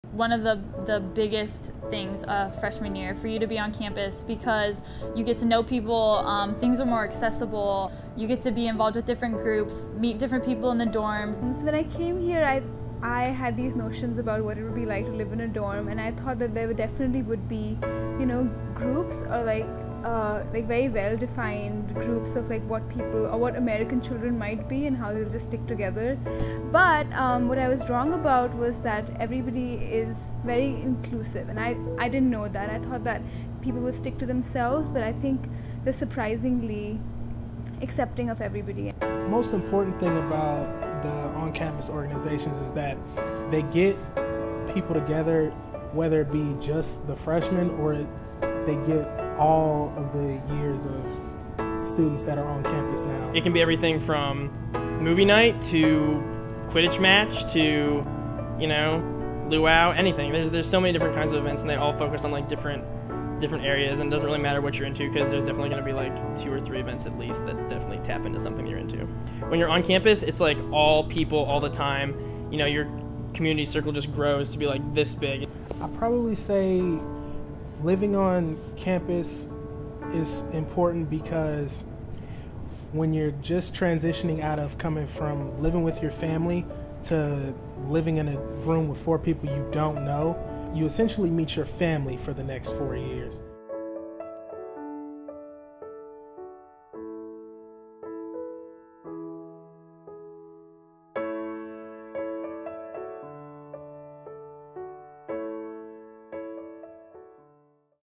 What is life like on CCAD's campus?
Interested to know what it's like to live on CCAD's campus? From student groups to campus living, see what it's like to be a part of the CCAD creative community.

Want to know more about CCAD?